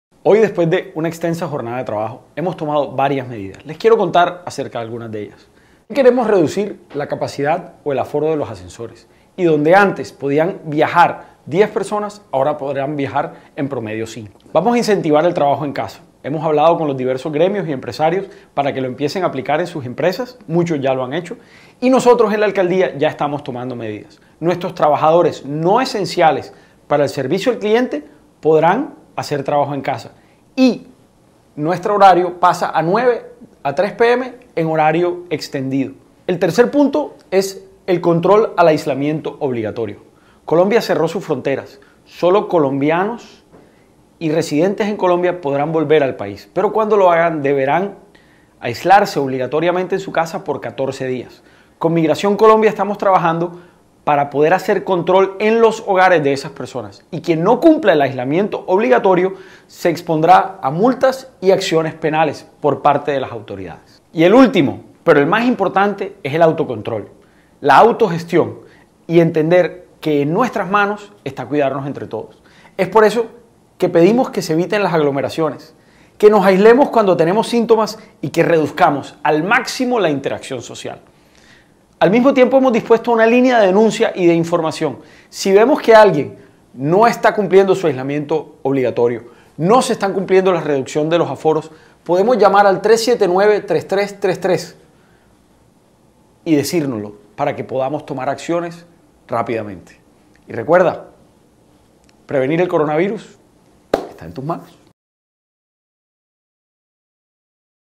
Estas son algunas medidas preventivas para combatir el coronavirus en Barranquilla (lengua de señas)
Si nos comprometemos con medidas preventivas podremos derrotar el coronavirus, ¡no bajemos la guardia!